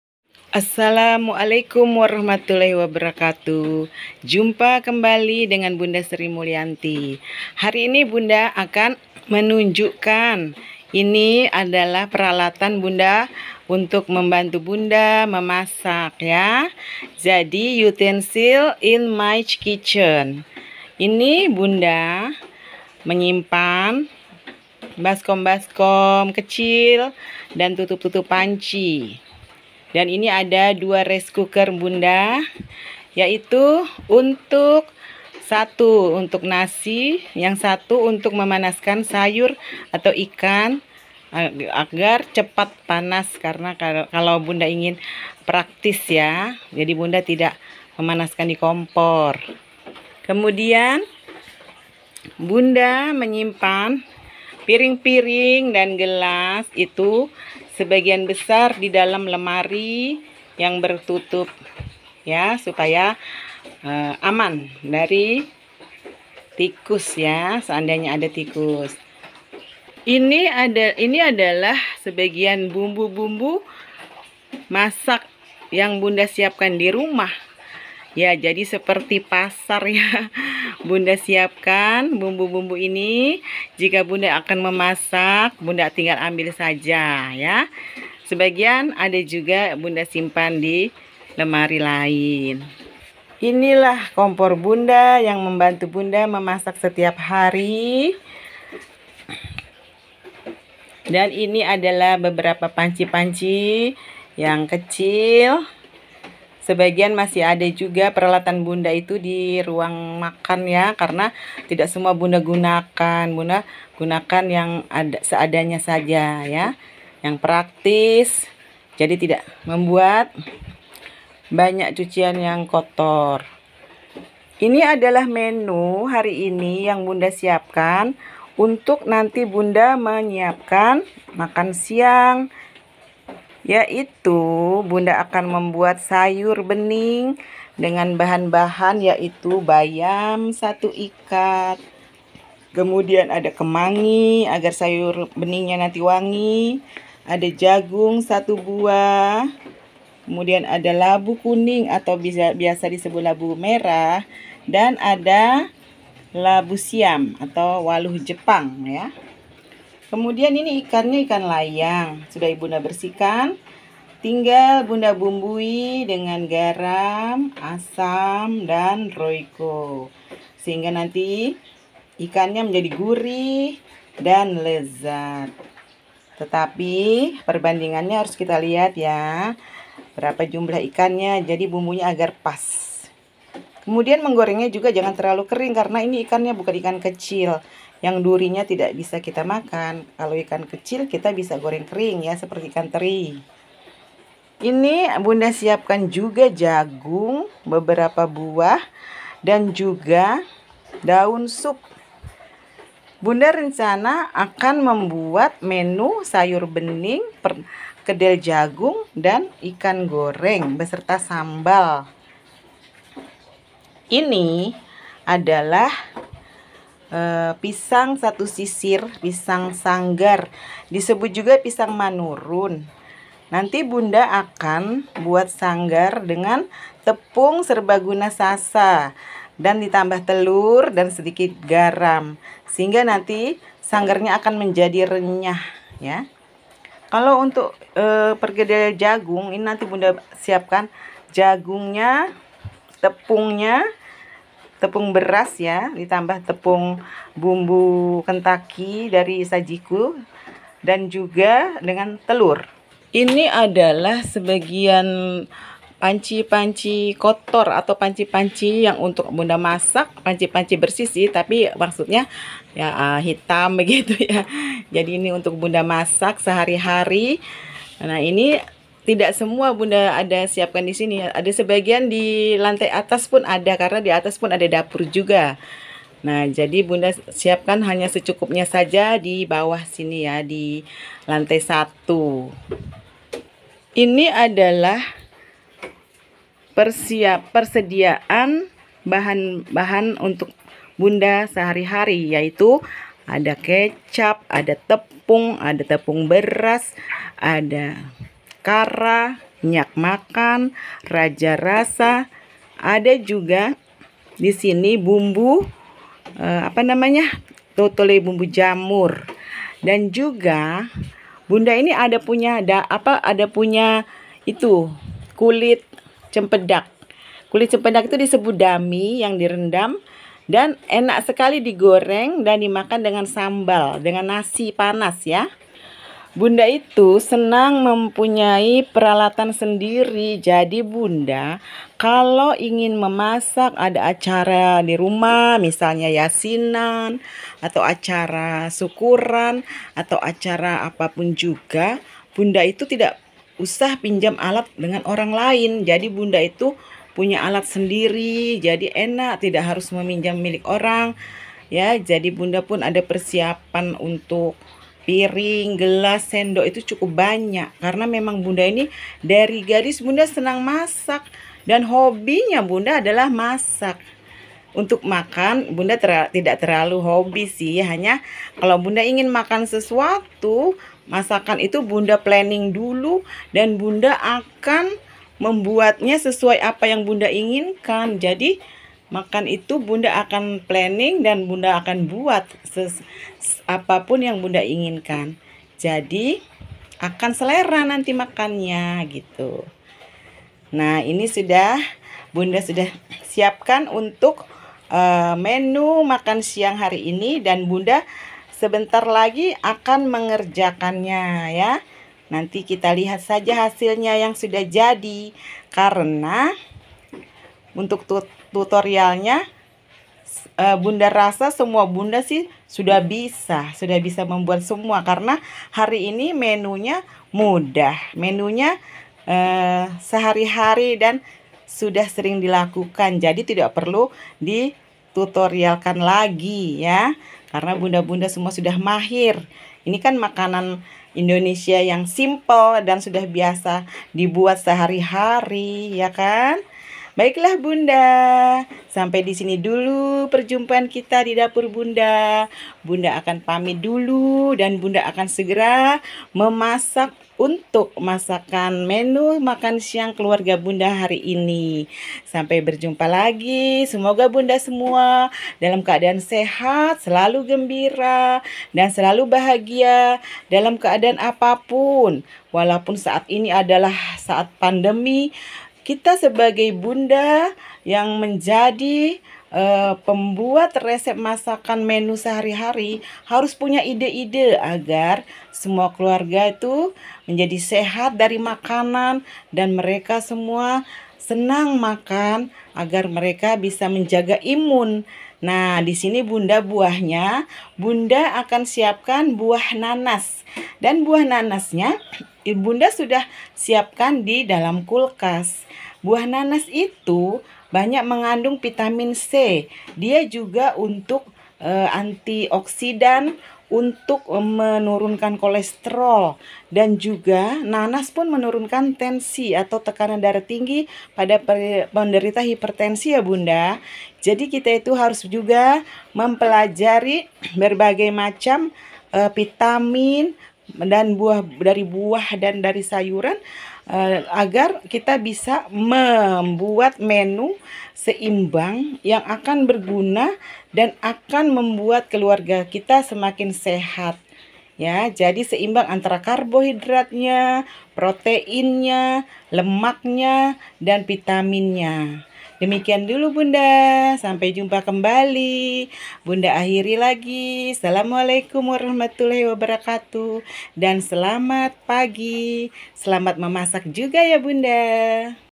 KITCHEN TOUR ...DI DAPUR BUNDA SRI MULIANTI....DAN BAHAN MENU BUNDA HARI INI...
bunda mnyispkandidapur apa saja bahan yg akan bunda masak untuk menu makan siang hari ini...